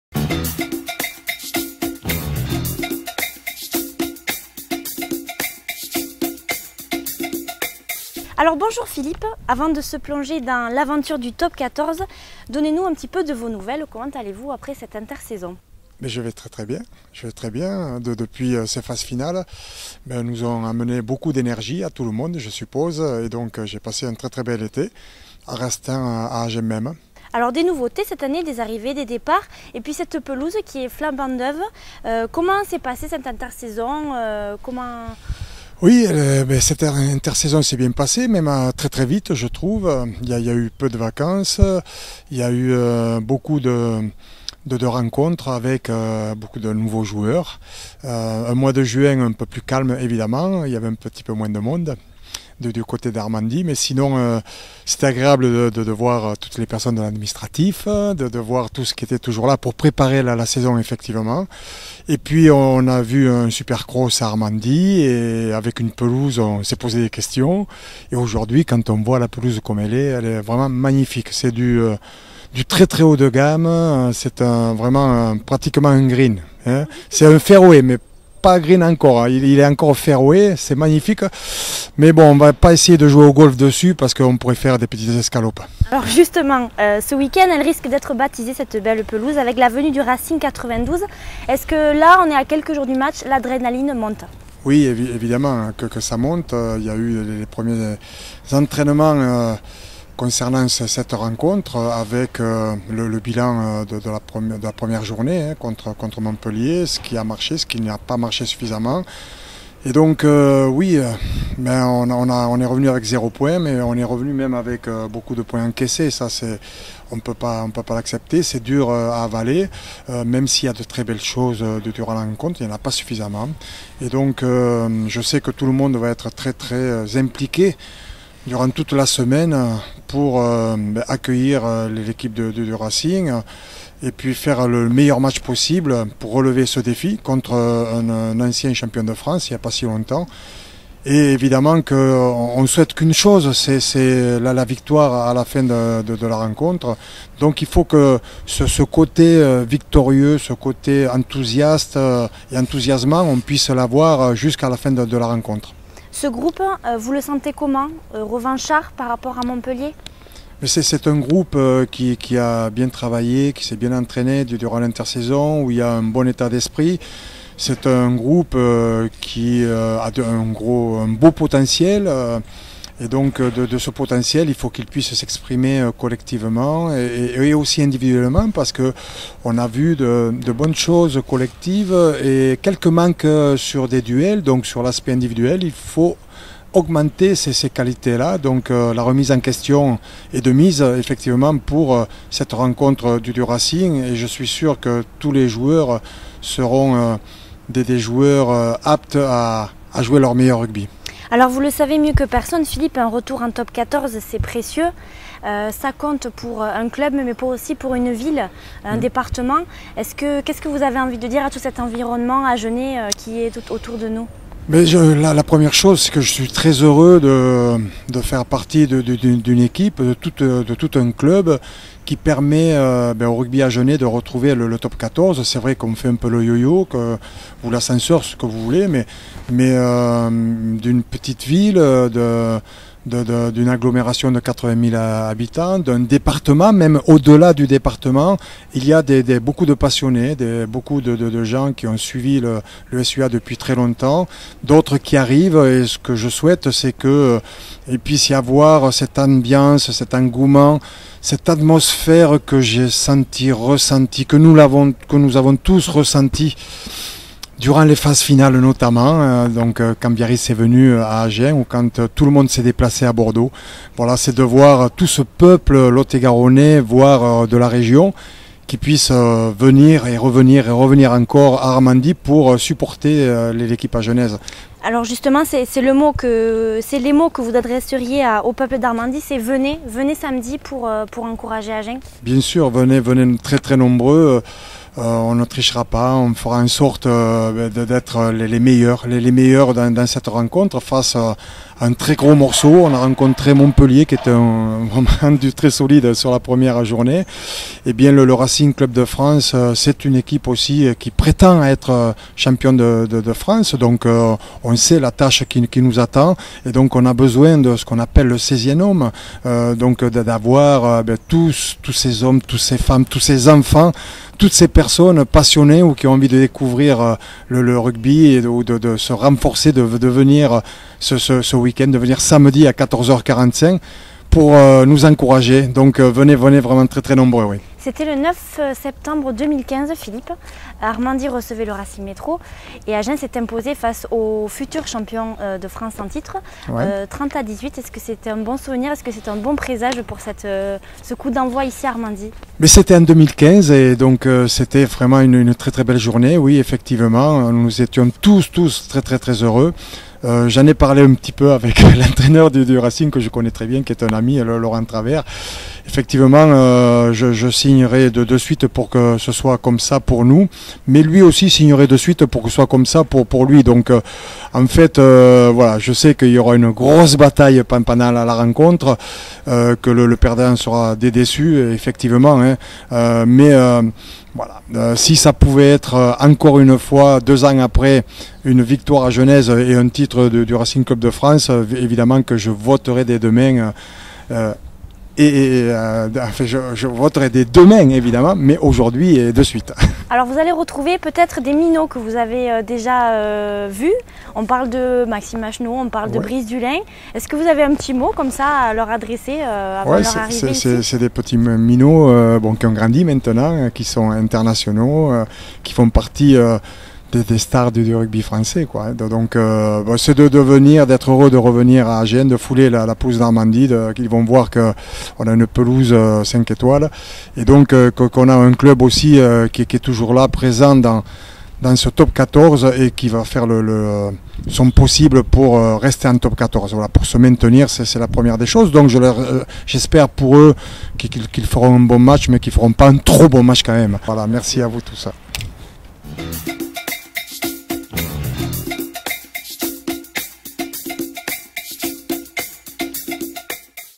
SUAR92 : Rencontre avec Philippe Sella
Semaine importante du côté d'Armandie qui reçoit son premier match de la saison 17/18 version TOP14 ! C'est l'Incomparable Philippe Sella qui nous parle de cette rencontre de haut vol. Rencontre.